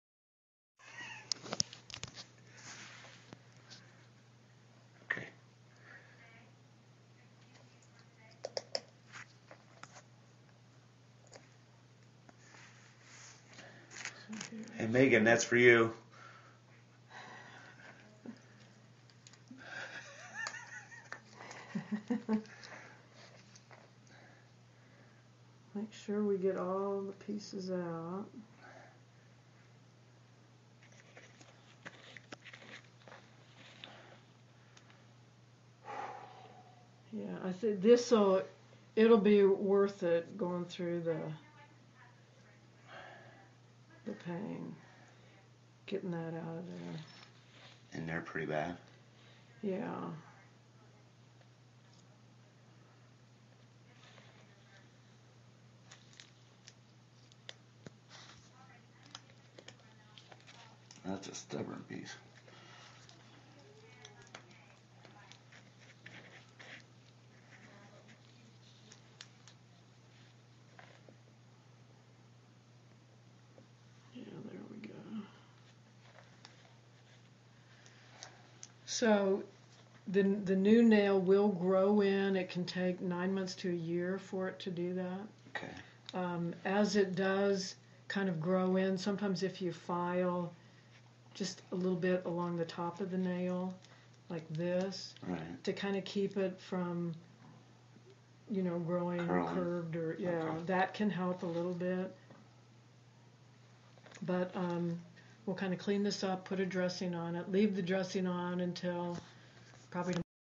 Gruesome not for weak stomach.
Removal of infected toenail, it took 8 shots to get it completely numb then they jerked the nail off. Notice the two handed grip to get the final part of the nail.